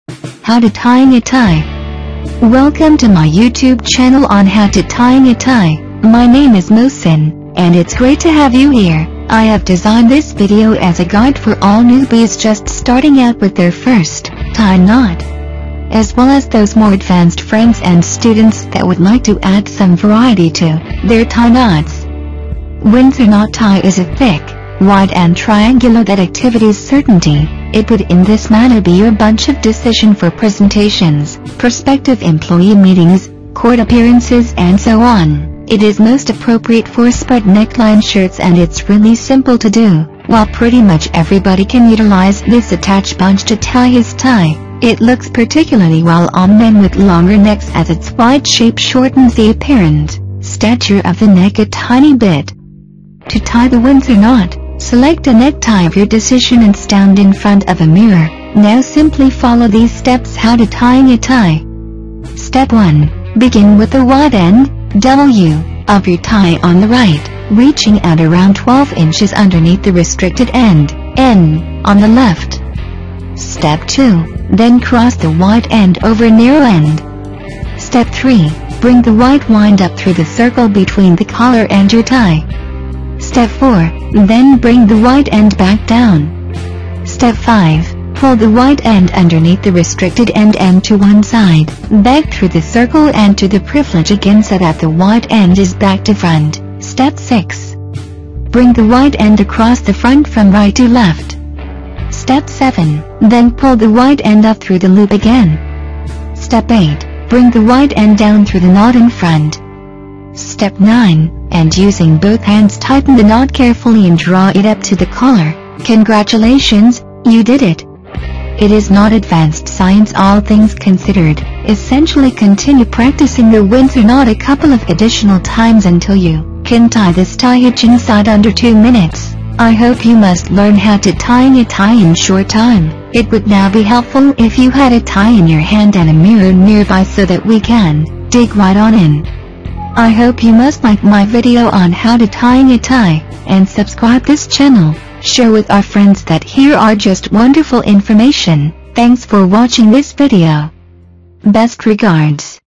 How To Tying A Tie - Windsor Knot Tai
How To Tying A Tie

to have you here. I have designed this video as a guide for all newbies just starting

out with their first tie knot, as well as those more advanced friends and students that

would like to add some variety to their tie knots.

Windsor Knot tie is a thick, wide and triangular that activities certainty. It would in

this manner be your bunch of decision for presentations, prospective employee meetings,

court appearances and so on. It is most appropriate for spread neckline shirts and it's

really simple to do.

While pretty much everybody can utilize this attach bunch to tie his tie, it looks

particularly well on men with longer necks as its wide shape shortens the apparent

stature of the neck a tiny bit.

To tie the Windsor Knot, select a necktie of your decision and stand in front of a

mirror. Now simply follow these steps how to tying a tie:

Step 1.
Begin with the wide end ("W") of your tie on the right, reaching out around 12 inches

underneath the restricted end ("N") on the left.

Step 2.
Then cross the wide end over narrow end.

Step 3.
Bring the wide wind up through the circle between the collar and your tie.

Step 4.
Then bring the wide end back down.

Step 5.
Pull the wide end underneath the restricted end and to one side, back through the circle

and to the privilege again so that the wide end is back to front.

Step 6.
Bring the wide end across the front from right to left.

Step 7.
Then pull the wide end up through the loop again.

Step 8.
Bring the wide end down through the knot in front.

Step 9.
And -- using both hands -- tighten the knot carefully and draw it up to the collar.

Congratulations, you did it! It is not advanced science all things considered.

Essentially continue practicing the Windsor Knot a couple of additional times until you

can tie this tie hitch inside under two minutes.

I hope you must learn how to tying a tie in short time. It would now be helpful if you

had a tie in your hand and a mirror nearby so that we can "dig right on in".

with our friends that here are
Best regards.